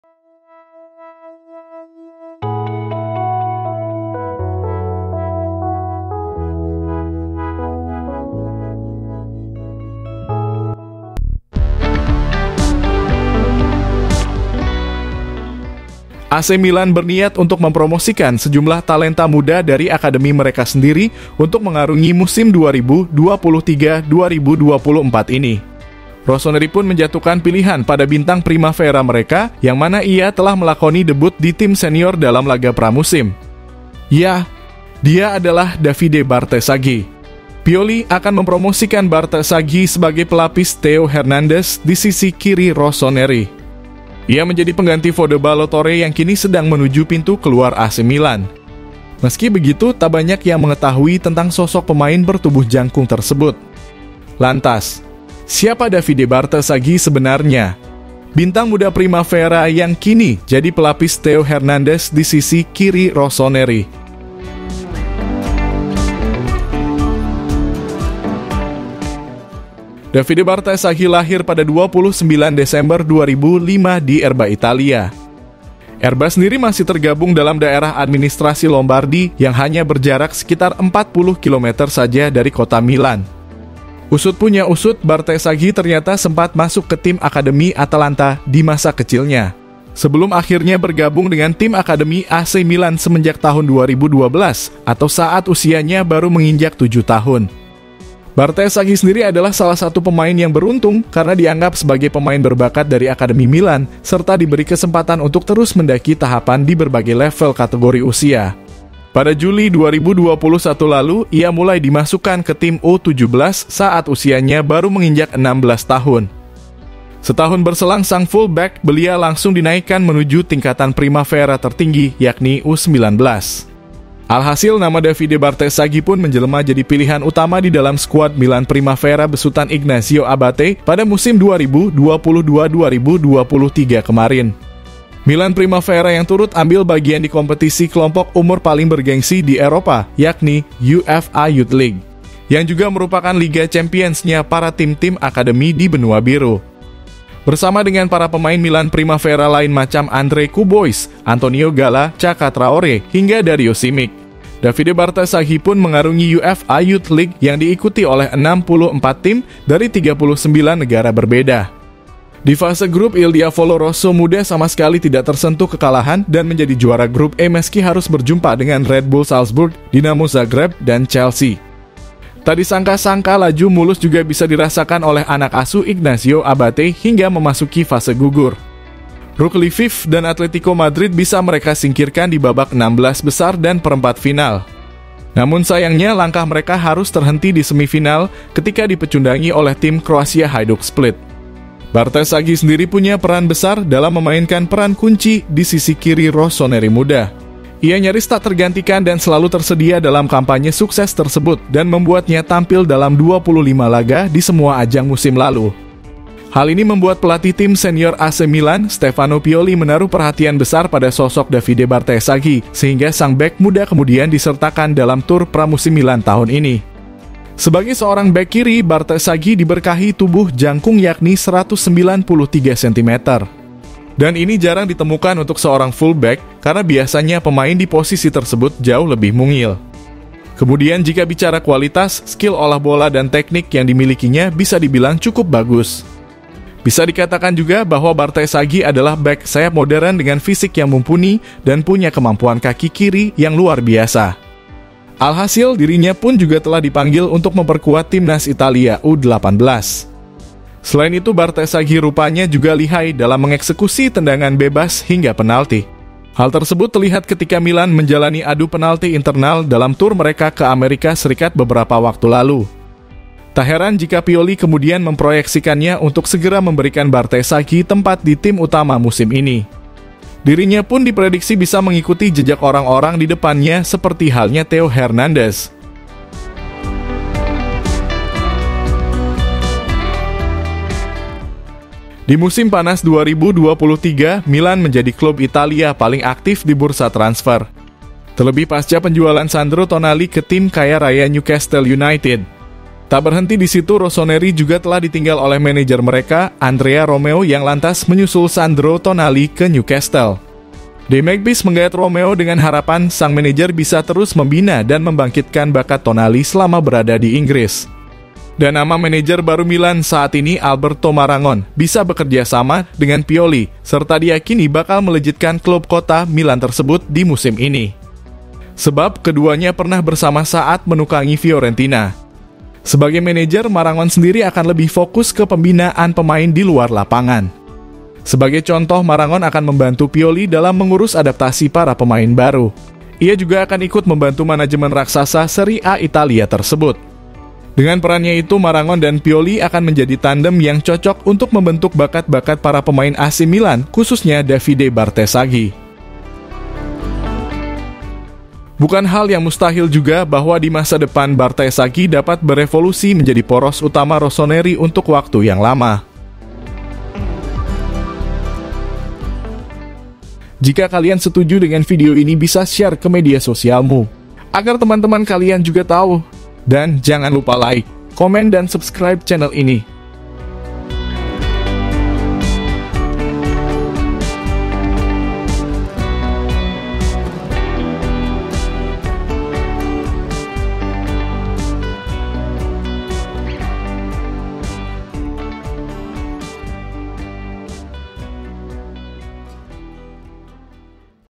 DARI PEMAIN MUDA AKADEMI JADI PELAPIS THEO HERNANDEZ DI AC MILAN, SEHEBAT APA DAVIDE BARTESAGHI?
AC MILAN BERNIAT UNTUK MEMPROMOSIKAN SEJUMLAH TALENTA MUDA DARI AKADEMI MEREKA SENDIRI, UNTUK MENGARUNGI MUSIM 2023/2024 INI. ROSSONERI PUN MENJATUHKAN PILIHAN PADA BINTANG PRIMAVERA MEREKA, YANG MANA IA TELAH MELAKONI DEBUT DI TIM SENIOR DALAM LAGA PRAMUSIM. YA. DIA ADALAH DAVIDE BARTESAGHI. STEFANO PIOLI AKAN MEMPROMOSIKAN BARTESAGHI SEBAGAI PELAPIS THEO HERNANDEZ DI SISI KIRI ROSSONERI. IA MENJADI PENGGANTI FODE BALLO TOURE YANG KINI SEDANG MENUJU PINTU KELUAR AC MILAN. MESKI BEGITU, TAK BANYAK YANG MENGETAHUI TENTANG SOSOK PEMAIN BERTUBUH JANGKUNG TERSEBUT. LANTAS. SIAPA DAVIDE BARTESAGHI SEBENARNYA. BINTANG MUDA MILAN PRIMAVERA YANG KINI JADI PELAPIS THEO HERNANDEZ DI SISI KIRI ROSSONERI. SIMAK VIDEO LENGKAPNYA !